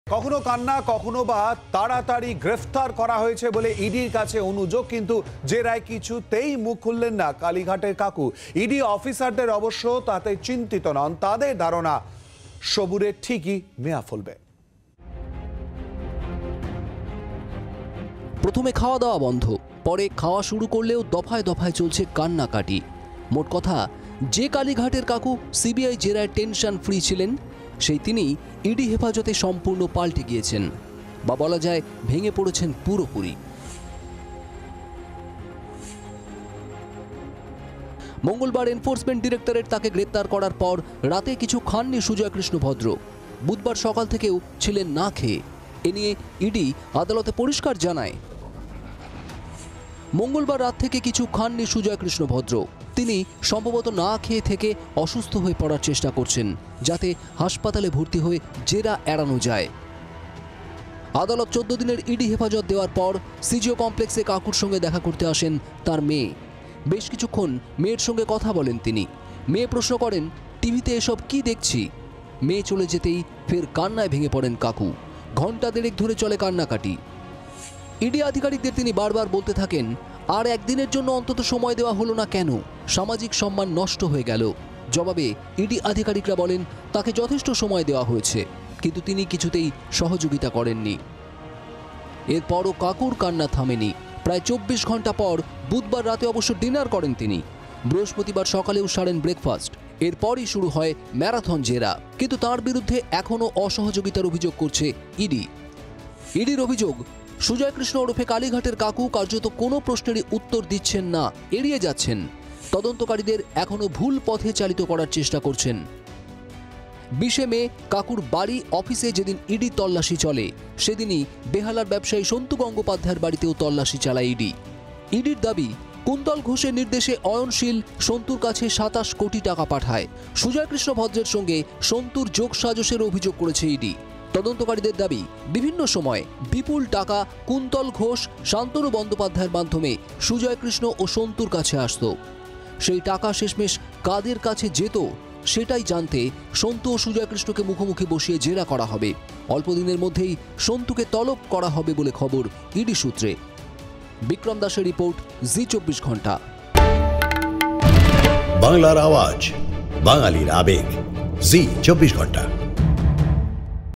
SSC Scam 2023: কান্না থামিয়ে উপবাস ভাঙলেও ইডি জেরায় নিরুত্তর সুজয়কৃষ্ণ ভদ্র! | Zee 24 Ghanta
SSC Scam: কান্না থামিয়ে উপবাস ভাঙলেও কার্যত অসহযোগের পথেই সুজয়কৃষ্ণ, ইডি জেরায় নিরুত্তর! | Zee 24 Ghanta